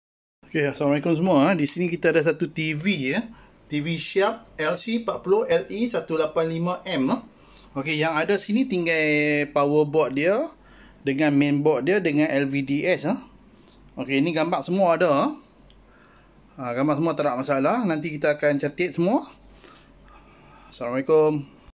Testing tv Sharp for sale at Shopee.
Testing tv Sharp LC-40LE185M for sale at Shopee.

Model TV  : Sharp LC-40LE185M

Main process : Notebook DELL LATITUDE E7240
Video by : Honor 20
Video Software Edit : Wondershare Filmora9
Picture profile editor : Wondershare Filmora9
Sebarang input atau pertanyaan boleh komen di bawah.

"Sebaik baik manusia, adalah manusia yang memberi manfaat kepada orang lain."